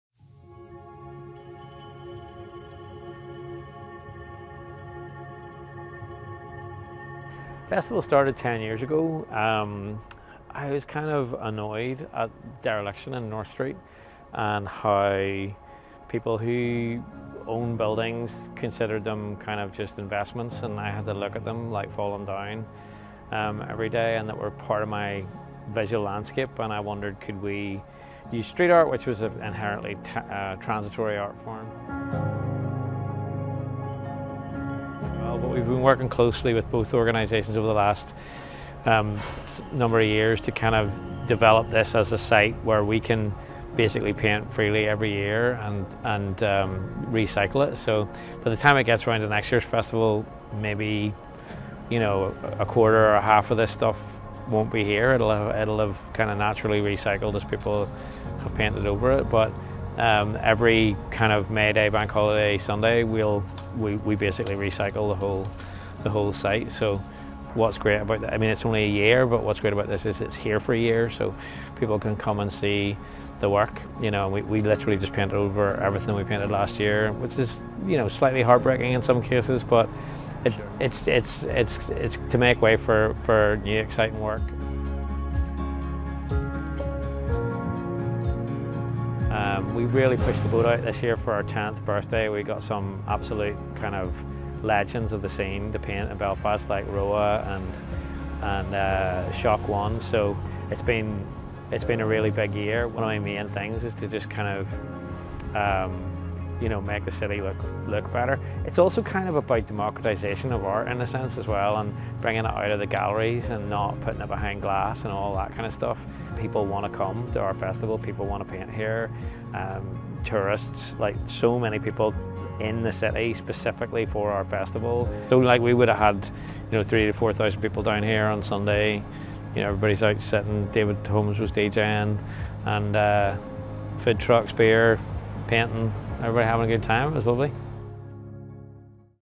Over 75 artists create new Belfast murals in festival celebration
Vibrant new murals have been added to the walls of Belfast by local and international artists thanks to the annual ‘Hit The North’ street art festival.
Celebrating its 10th birthday, the festival drew its most impressive crowd yet with over 1,000 spectators attending the annual ‘paint-jam’ in the city centre to see artists including Studio Giftig, winner of this year’s Street Art Cities ‘Best Mural of the 2022’ Award.

Video: Aodhan Roberts/Belfast Telegraph.